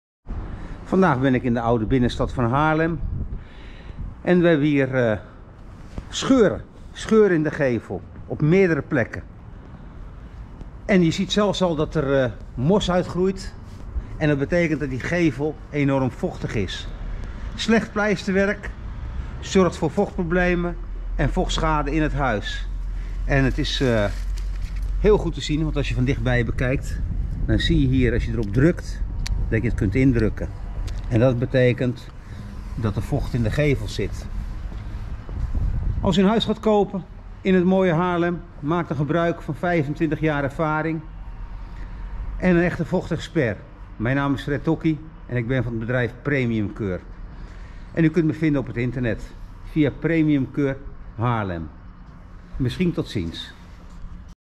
Vochtinspectie Haarlem
Vochtinspectie Haarlem. In Haarlem komen veel vochtproblemen voor die ernstige schade aan uw huis kunnen veroorzaken. Bekijk deze leerzame video